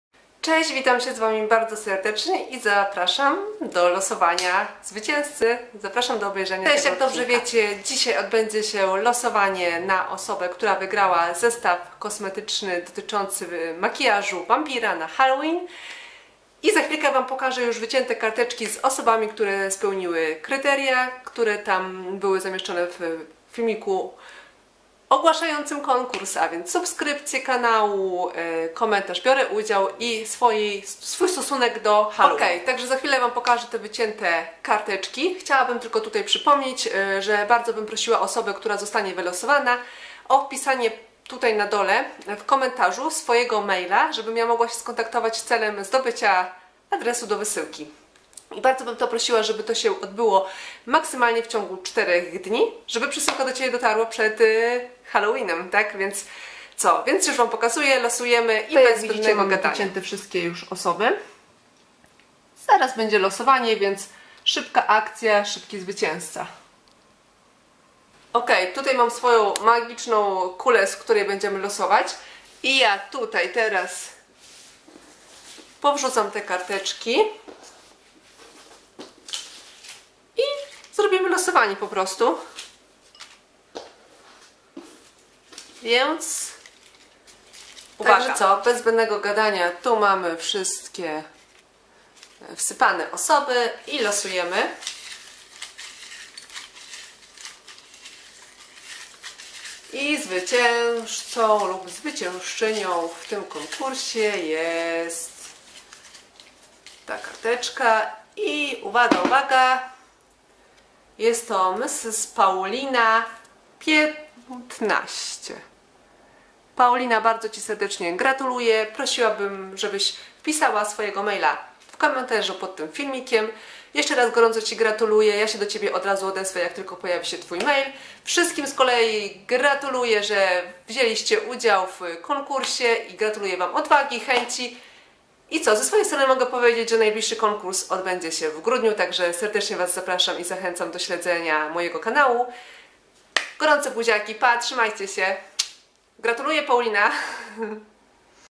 Wyniki konkursu HALLOWEEN 2018 :)
Witam,

Zapraszam na szybkie losowanie zwyciężcy! Wygrana-Kosmetyki na Halloween :)

Osobie wylosowanej gratuluję i czekam na komentarz z e-mailem żebym mogła się skontaktować!:) Pozostałym bardzo dziękuję za udział, w grudniu kolejny konkurs!

Buziaki, Ala :)